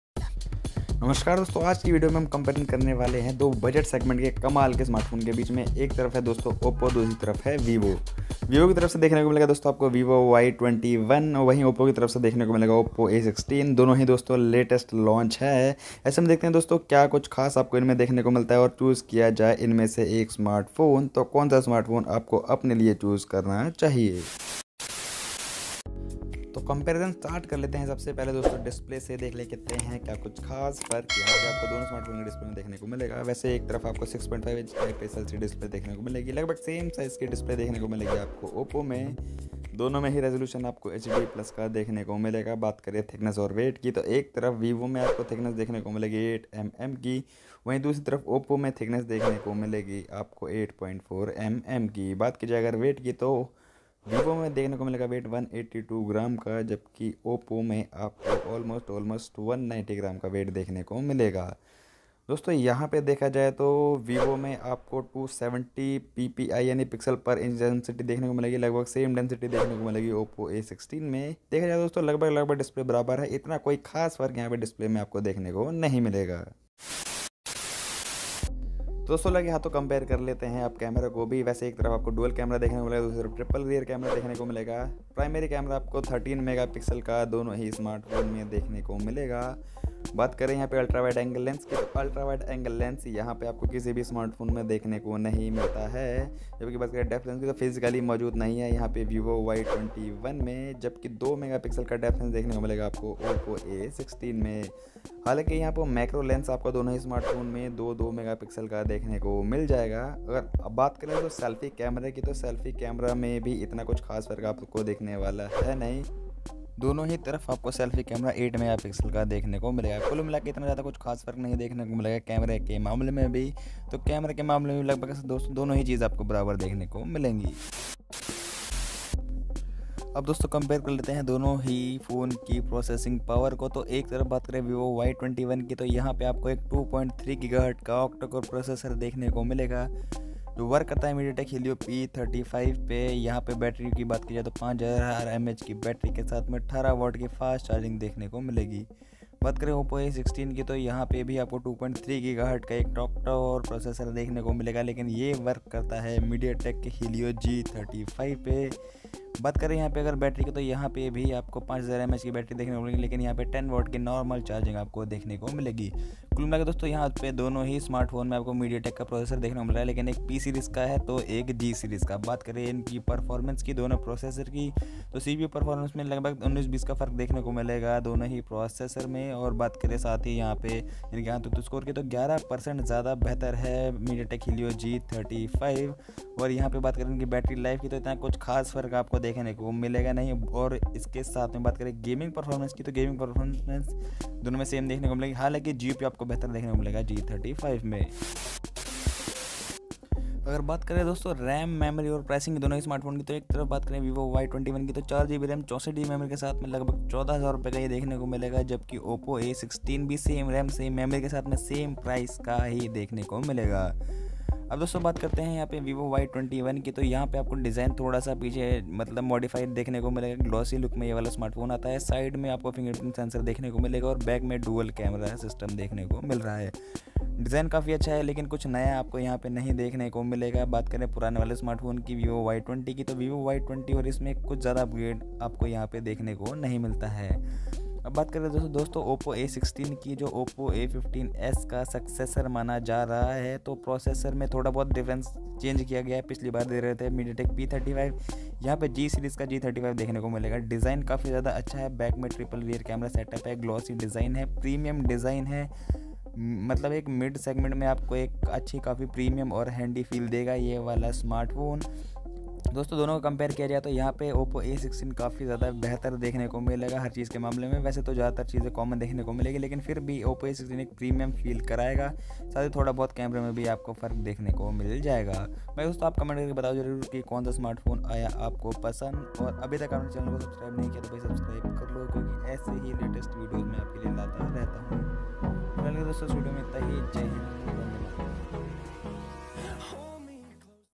OPPO A16 vs VIVO Y21 | कौन है बेहतर? | hindi comparison | Mediatek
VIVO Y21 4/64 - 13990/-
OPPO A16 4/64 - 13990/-

नमस्कार दोस्तों आज की VIDEO मे हम COMPARISON करेंगे VIVO Y21 का OPPO A16 के साथ मे  ! देखेंगे की कौन सा PHONE हो सकता है आपके लिए बेहतर फोन और कौनसा आपको लेना चाहिए। तो दोस्तों VIDEO को पूरा एंड तक देखना और अगर आपको VIDEO पसंद आए तो VIDEO को 1 लाइक ZARUR करना AND I होप आपको VIDEO पसंद आयेगी।